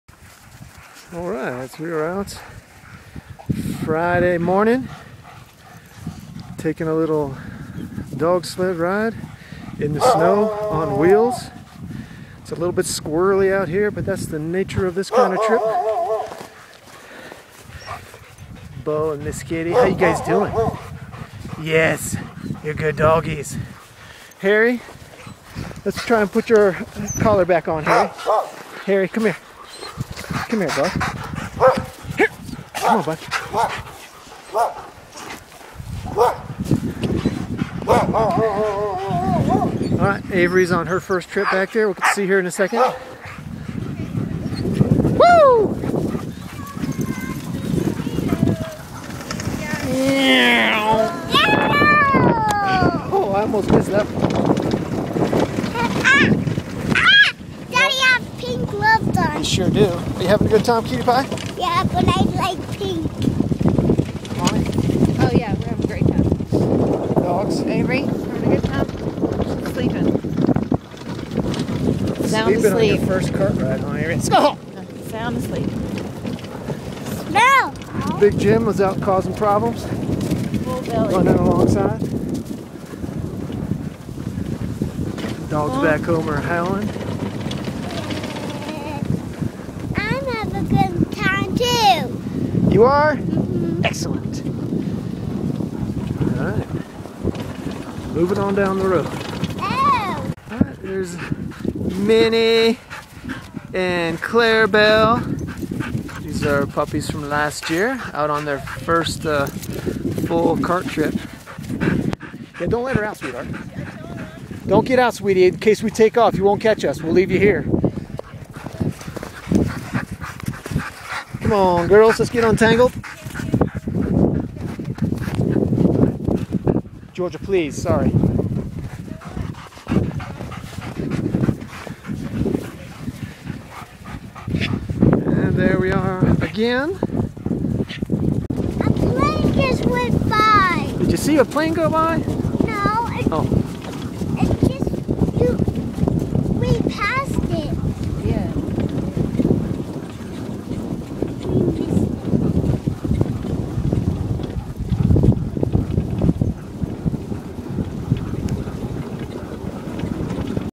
Dog Sled Ride in a Cart on Snow with Dog Sled Rides of Winter Park
Got the whole family out for a cart ride on 11/18/2011.  Not enough snow for a sled, but we can still have fun on wheels.  Twelve dog team of Alaskan Huskies. First full run for two puppies.